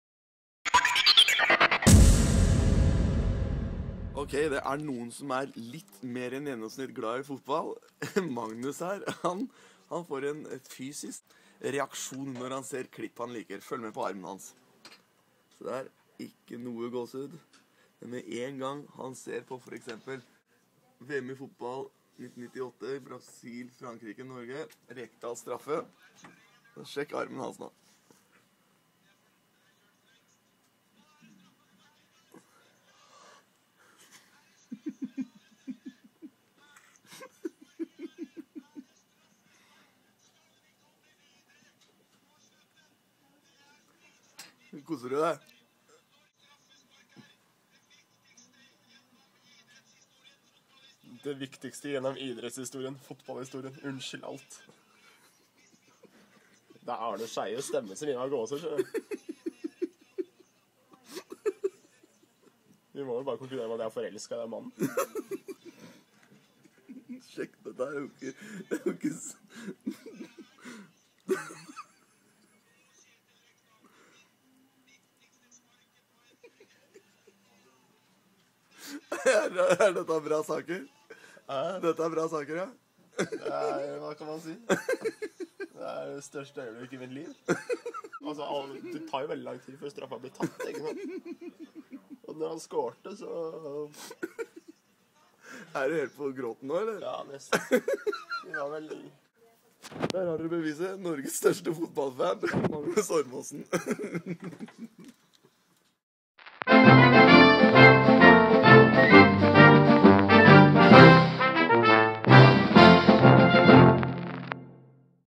Norges største fotballfan?
Pondus.nos medarbeider Madmag er LITT for glad i fotball, noe denne videoen tydelig viser. Sjekk hvordan han reagerer på å se videoklippet av Kjetil Rekdal som scorer mot brasil i Fotball-VM 1998...